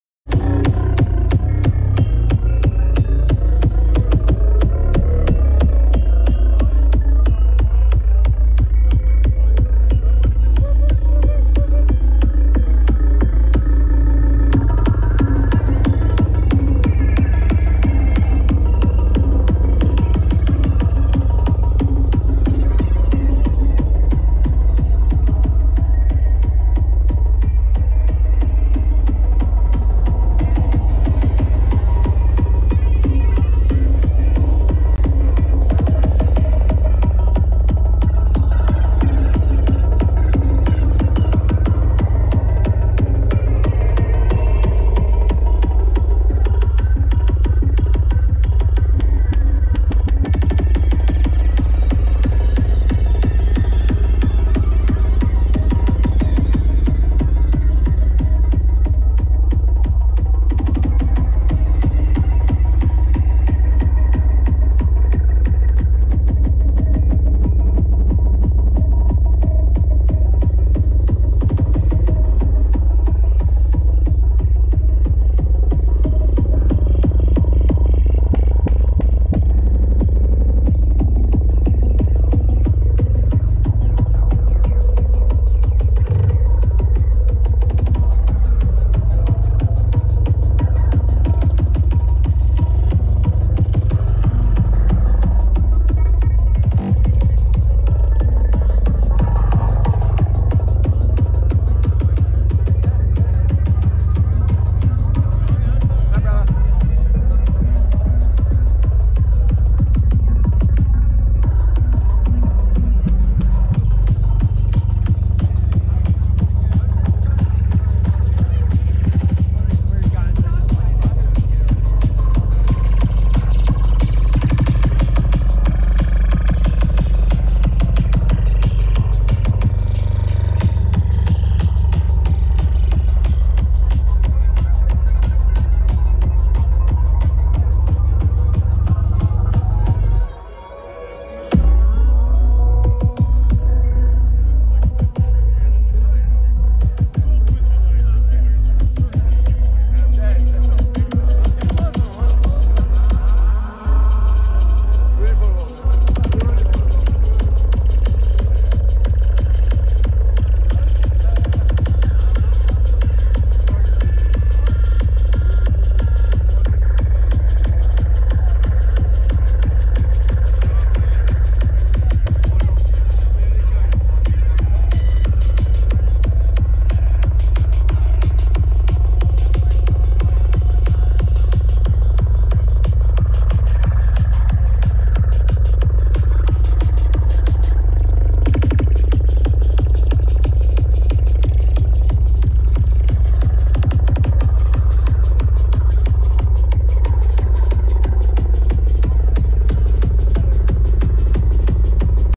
Uga-Booga - Starcamp Shasta @ Mount Shasta, California. July 5-7, 2024
Uga-Booga (Iwazaru Records) set at Starcamp Shasta with the home grown soundsystem and love tended land of the Starseed Psytrance crew. Hidden in the forests surrounding this majestic peak lies a gathering of tribes with a powerful intention and beautifully thought out decoration, altar and festival amenities. We were blessed with a late snow melt which resulted in a full lake to enjoy just a few meters from the dancefloor. A real treat to enjoy some water fun during a scorching heatwave in the mountains of California all the while listening to some top shelf music from across the world. It s difficult to describe just how powerful and transformational it is to dance on the side of such a powerful and sacred location of the world like Mount Shasta.

All videos are recorded in a live setting and performed by various artists in the global circuit.